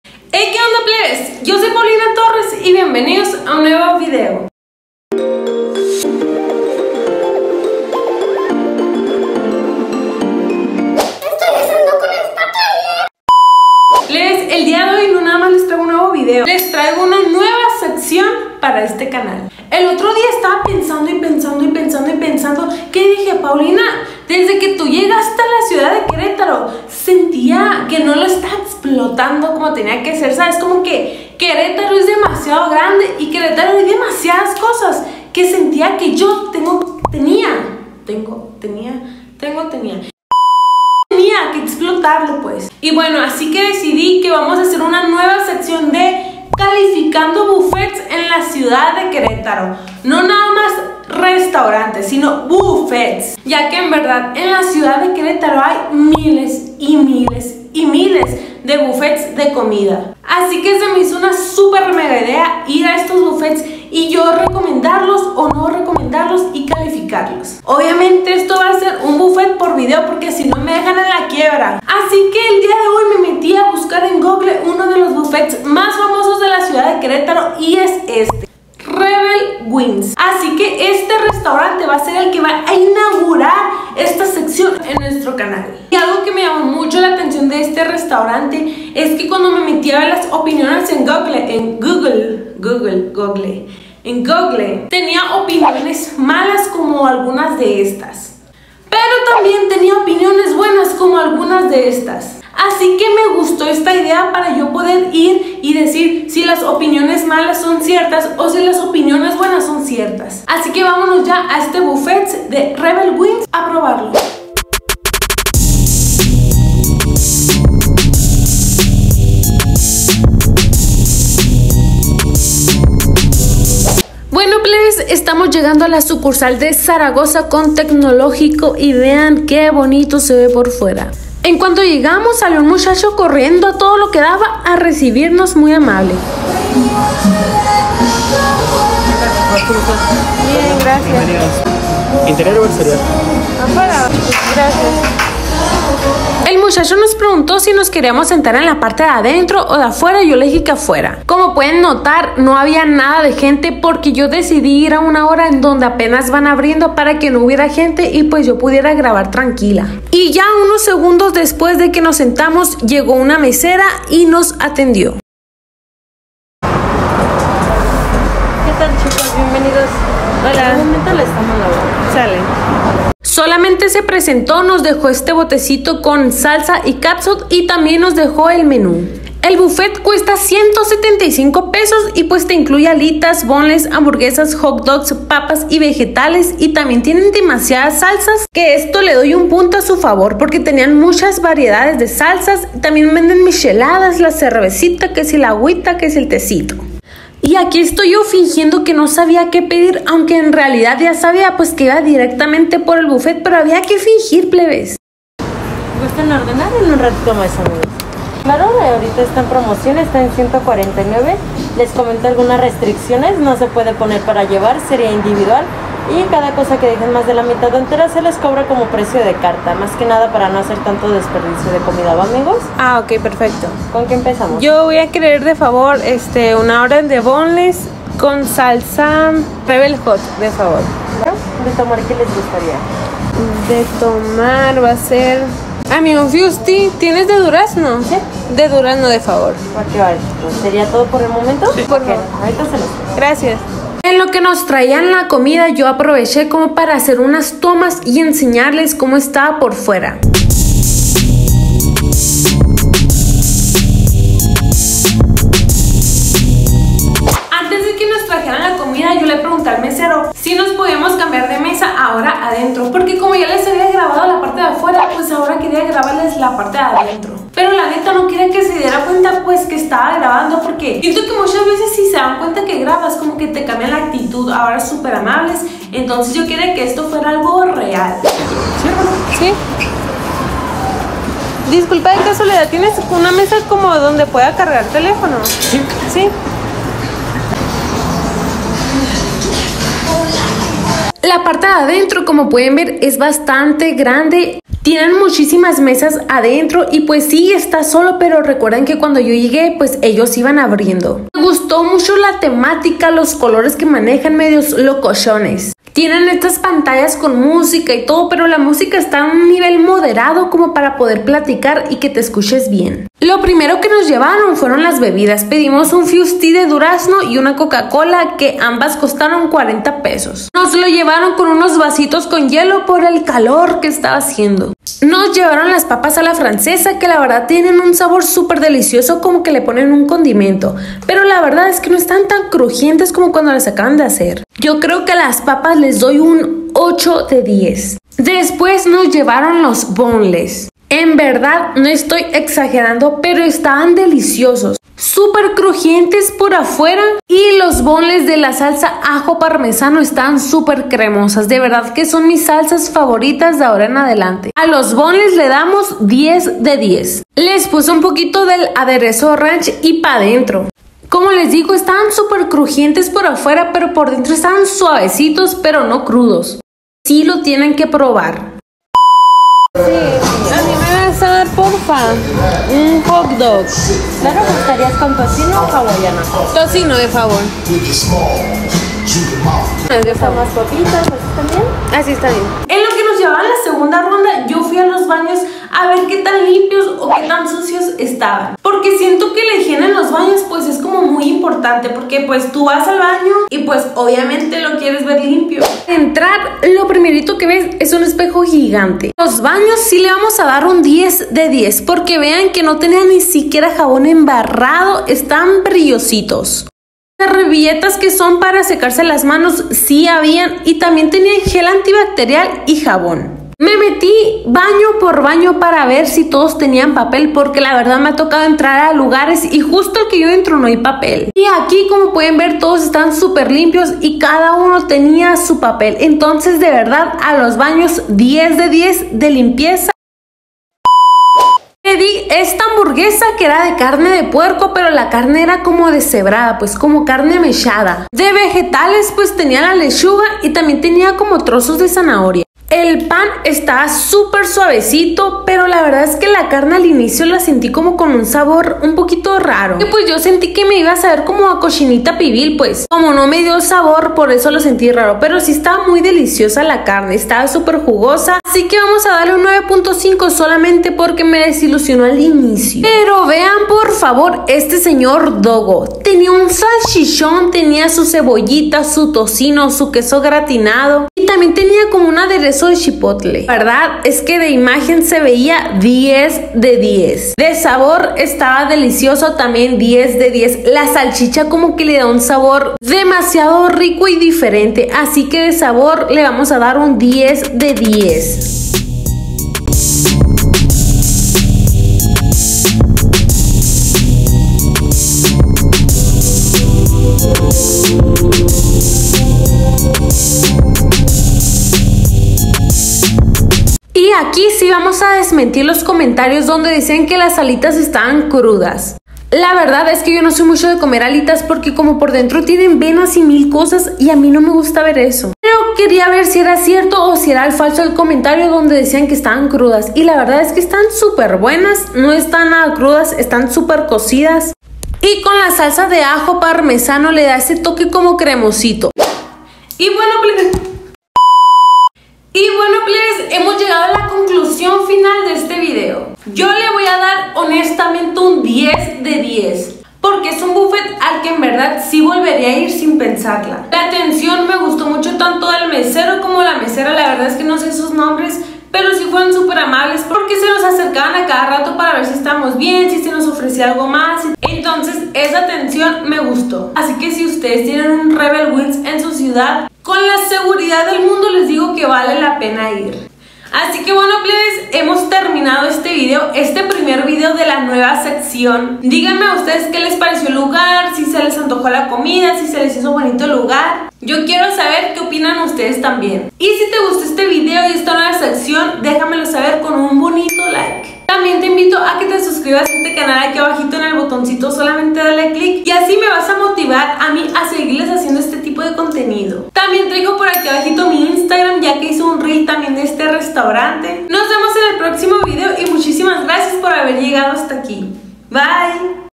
CALIFICANDO BUFFET en QUERÉTARO || REBEL WINGS
Hey Qué onda plebes! Yo soy Pautorresch 💖
Bienvenidos a esta nueva sección de CALIFICANDO BUFFETES EN QUERÉTARO!!

Este restaurante se llama Rebel wings y lo recomiendo 10/10!!✅👌🏻

Cuéntame que te pareció?🫶🏻👀

Te dejo mi Instagram por si gustas seguirme !💖🫶🏻